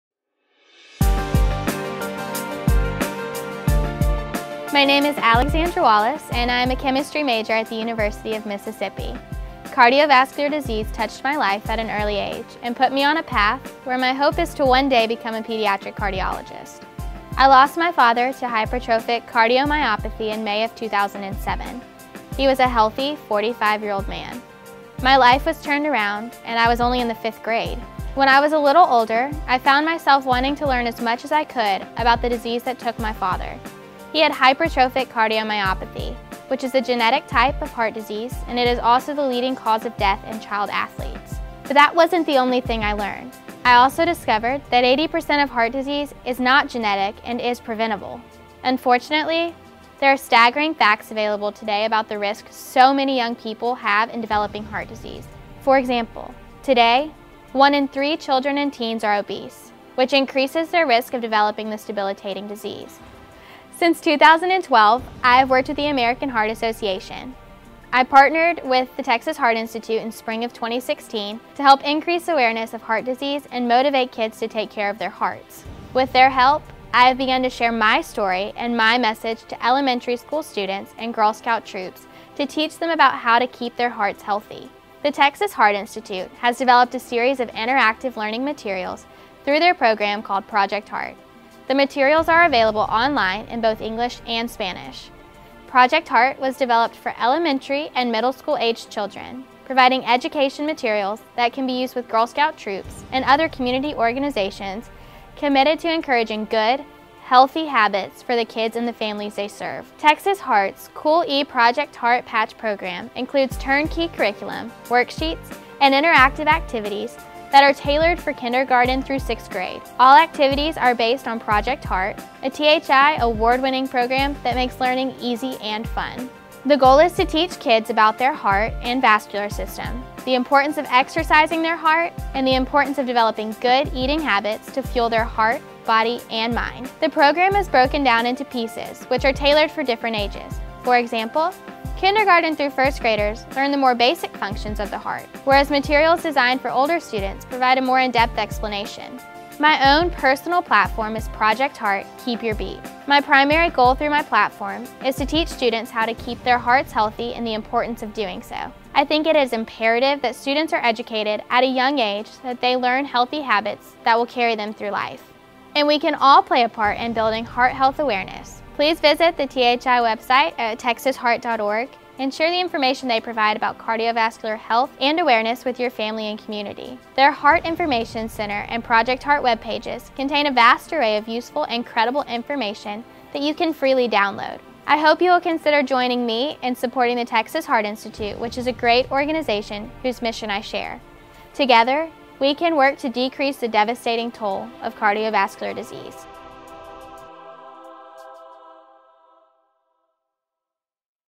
Alexandra Wallace for Project Heart: Keep Your Beat
Alexandra Wallace is a Texas Heart Institute Heart Health Ambassador and bio-chemistry major at the University of Mississippi. Cardiovascular disease touched her life at an early age when she lost her father to hypertrophic cardiomyopathy.  Today her mission is to teach students how to keep their hearts healthy and the importance of doing so through her own personal platform Project Heart: Keep Your Beat.